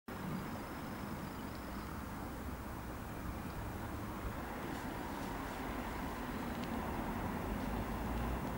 Přituhlo a zamrzlo.....
Ve čtvrtek 23.12. v Brně přituhlo a zamrzlo.